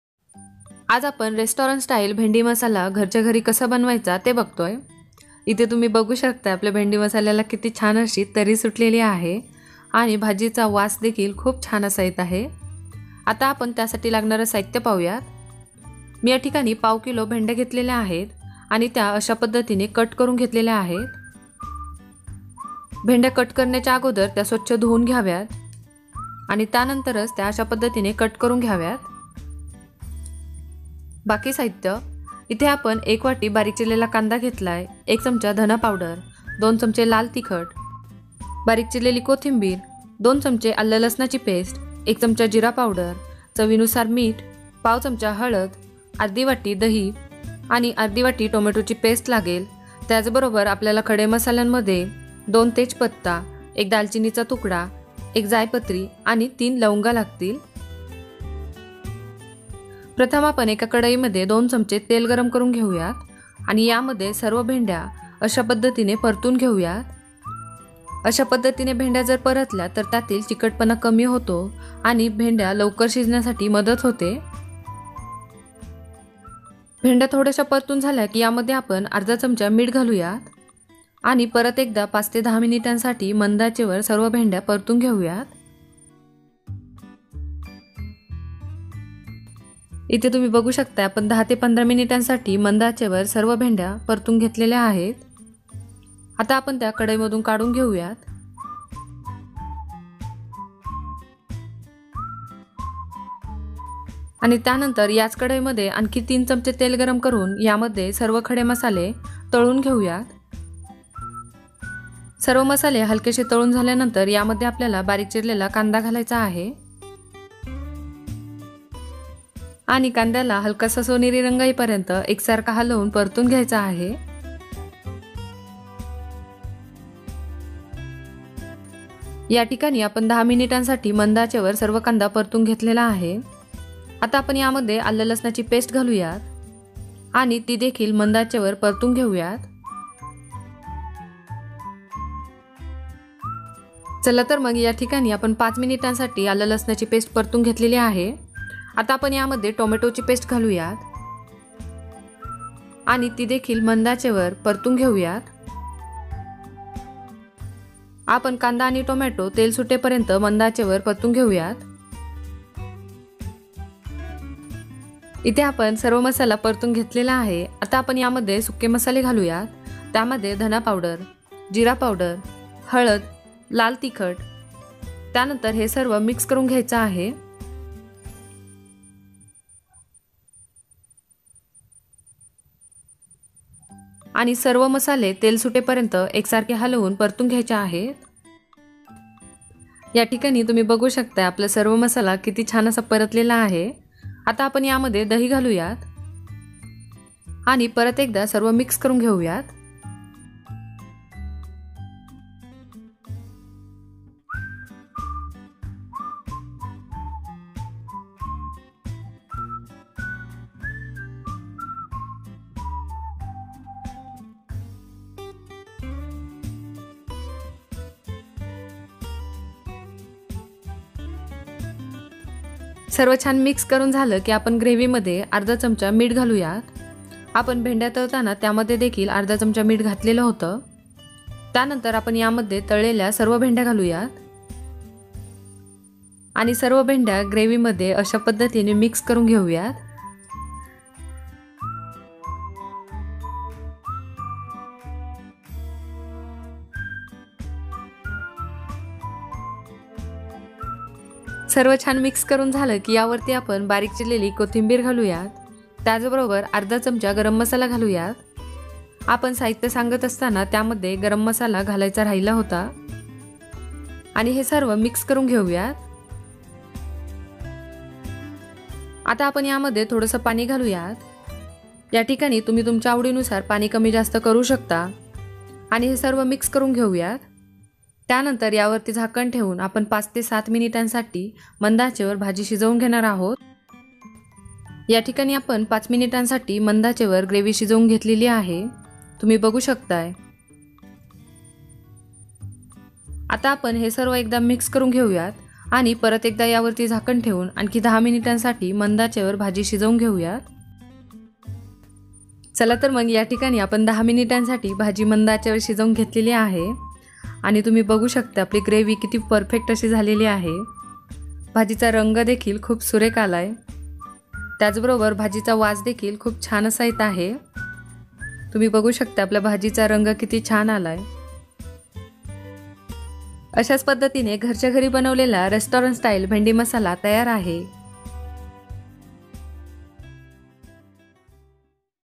अशी भेंडी जर बनविली तर एक ऐवजी दोन पोळ्या तर नक्कीच खाणार | भेंडी मसाला रेसिपी | Bhendi Masala
Ingredients :

● 250 gm Ladyfinger
● 1 Bowl Finely chopped onion
● Coriander Leaves
● 2 Tsp Garlic ginger paste
● 1/2 Bowl Curd
● 1/2 Bowl Tomato paste
●Whole spice (Bay leaf, Cinnamon, Clove, Mace)